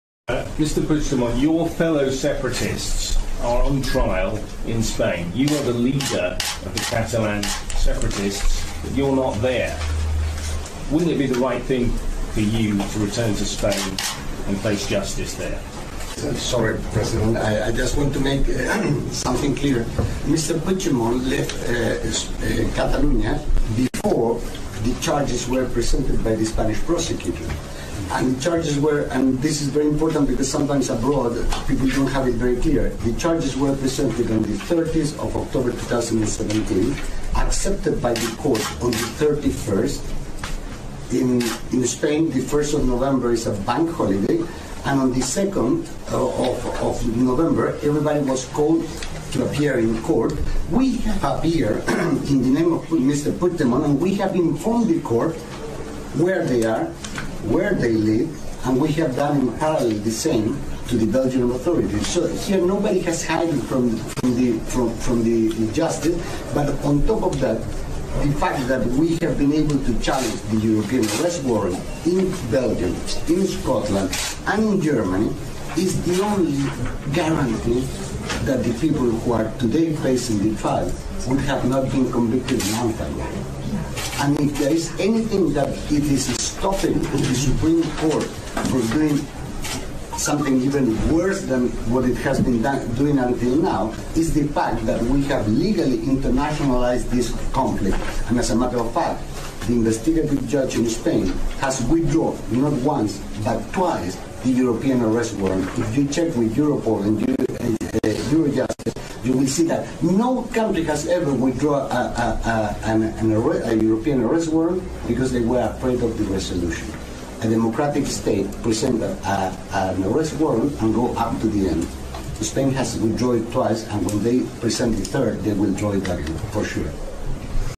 Gonzalo Boye explica por qué Puigdemont no es un fugado de la justicia
El abogado Gonzalo Boye, a una pregunta de un periodista sobre si Puigdemont debería volver a España y entregarse a la justicia, aclara cual es su situación y los motivos por los cuales no es correcto considerarlo fugado.